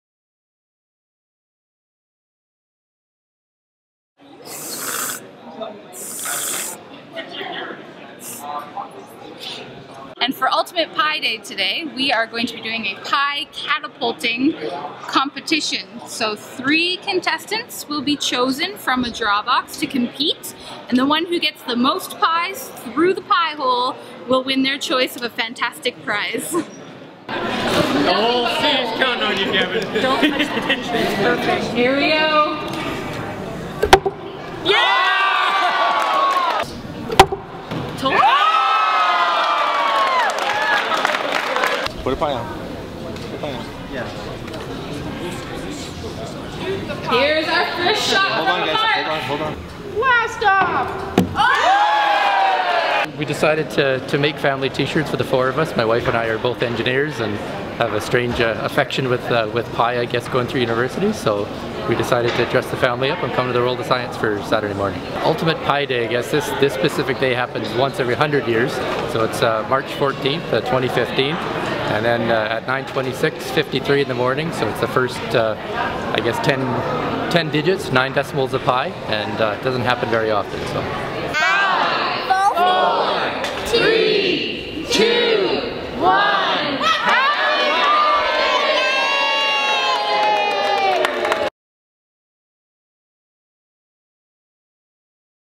Pie-catapult contest held on Ultimate Pi Day
The Telus World of Science held a pie-catapulting contest at exactly 9:26:53 a.m. on Saturday, March 14, 2015, to celebrate Pi Day, commemorating the mathematical constant. This year was dubbed the Ultimate Pi Day as the first 10 digits of Pi (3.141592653) occurred during the morning of March 14, 2015, or 3/14/15 at 9:26:53 a.m. Video by Larry Wong, Edmonton Journal